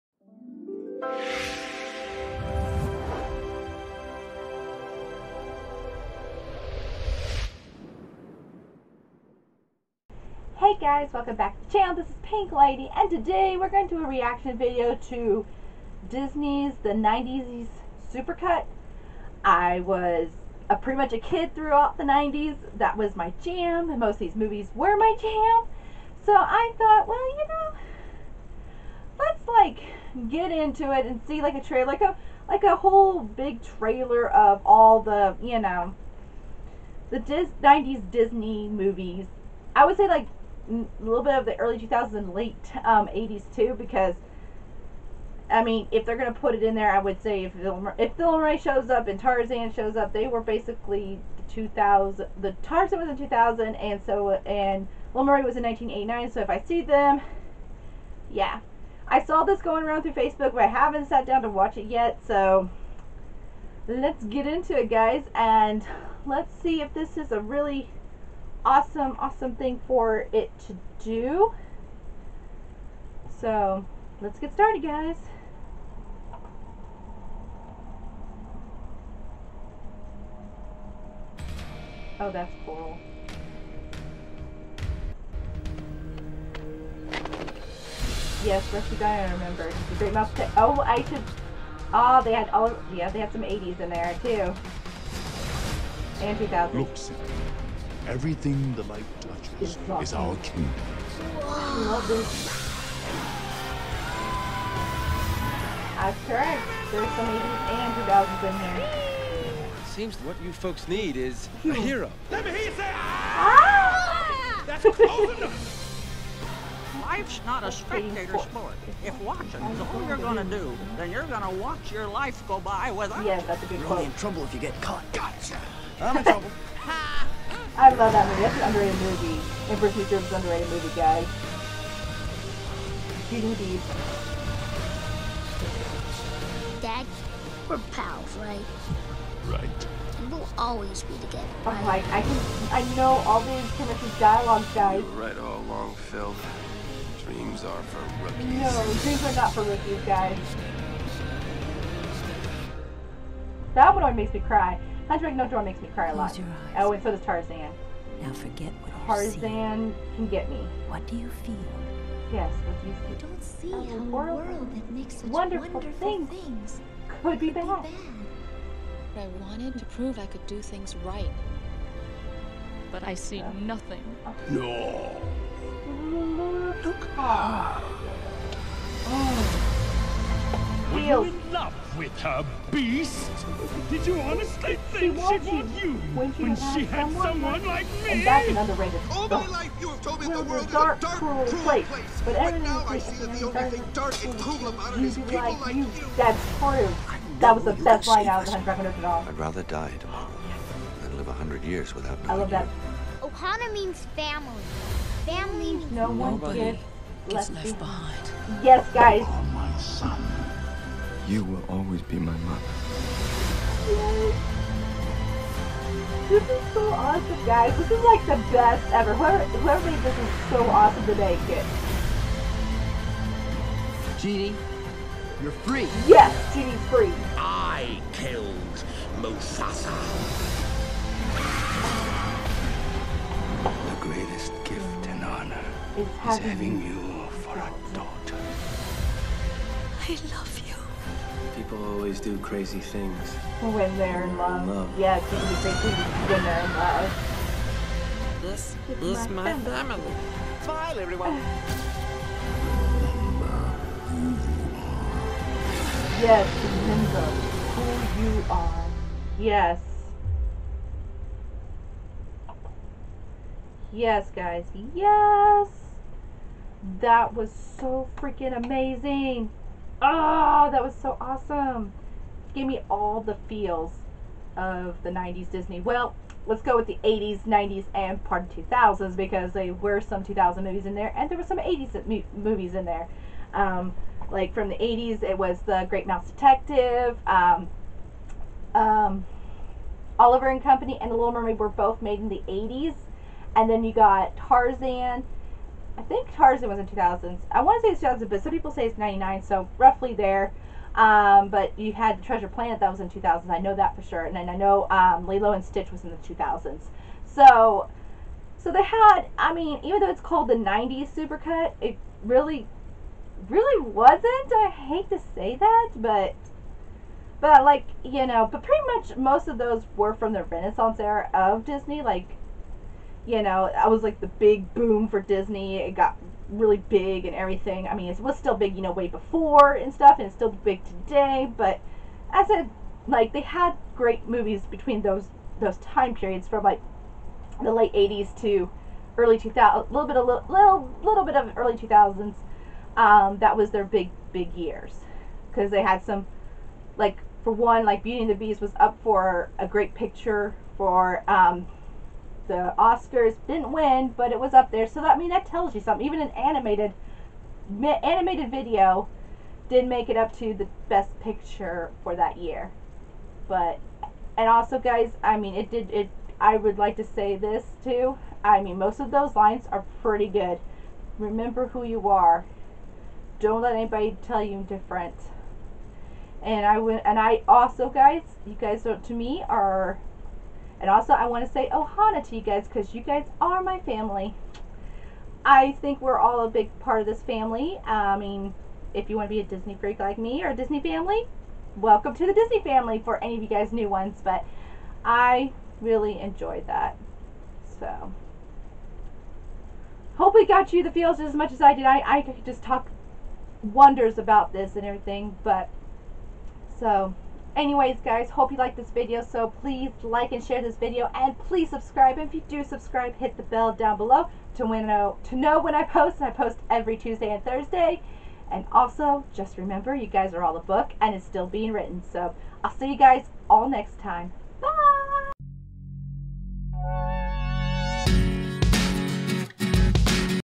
Best Disney Trailer!!!
I enjoyed watching the fan made trailer for all the Renaissance Disney!!! All of these movies I grew up with and still watch today!!! Also I appreciate you all for being curious or is a Disney Nerd!!!